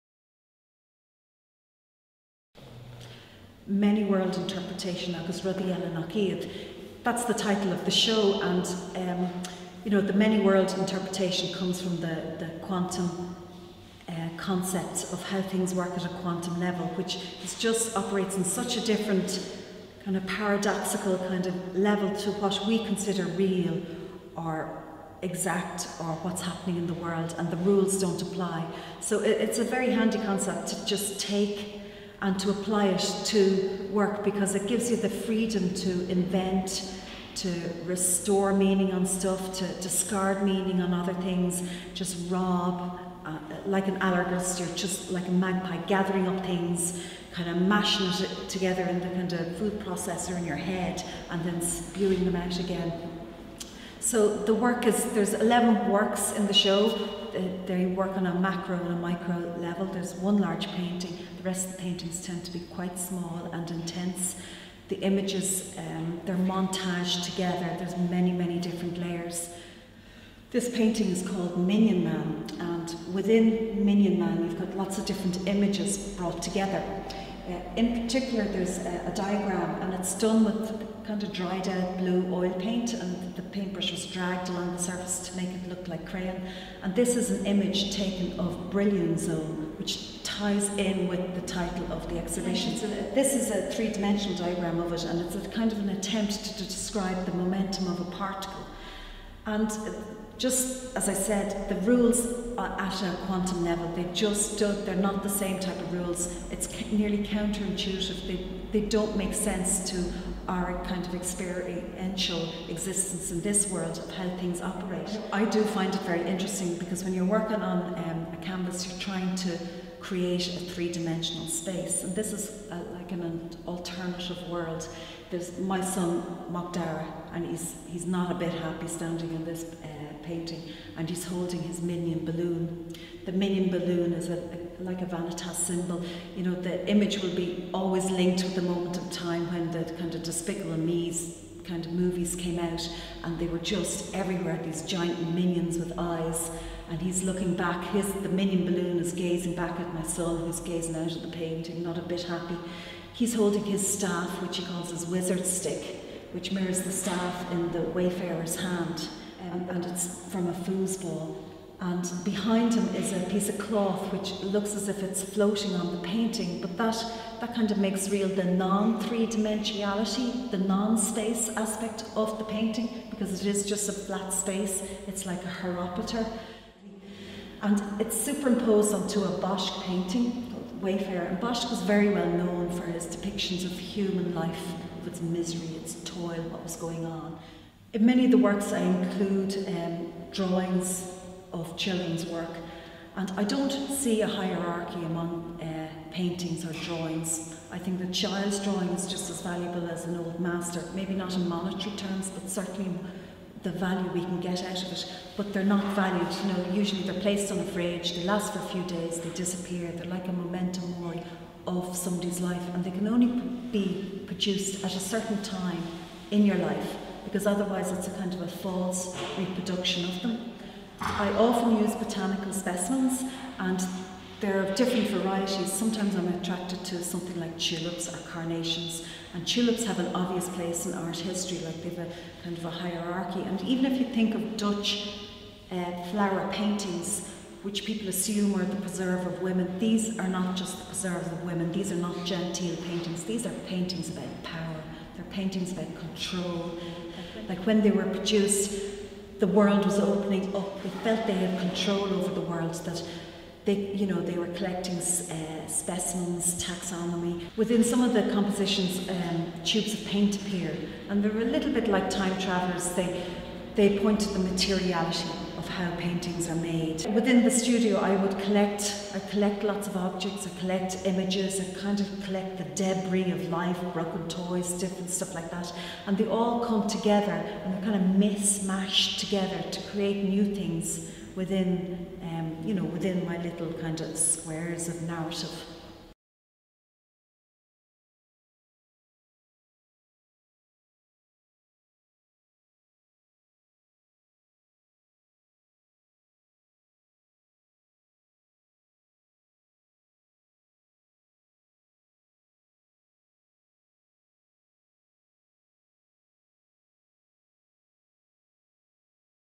KEVIN KAVANAGH - GERALDINE O'NEILL
GERALDINE O’NEILL
MANY-WORLDS INTERPRETATION…
16 FEBRUARY - 18 MARCH 201

Many-worlds interpretation agus rudaí eile nach iad is an exhibition of recent paintings by Geraldine O’Neill. Drawing on a wide variety of sources, she includes objects from within her household that have personal significance as well as images gleaned from the history of painting. Her emphatic use of children’s drawings, as well as the appearance of children as subjects within her compositions harks to the manner in which they come to terms with their world. Children often conflate playing, learning and dreaming and this is an important reference within the artist’s oeuvre. The content of O’Neill’s paintings constitute a collage through the history of image making as well as the personal history of the artist. These paintings are detailed and dense, replete with allegory and symbolism. Through her fascination with emblematic devices in northern renaissance painting O’Neill recalls motifs and iconography and considers them afresh.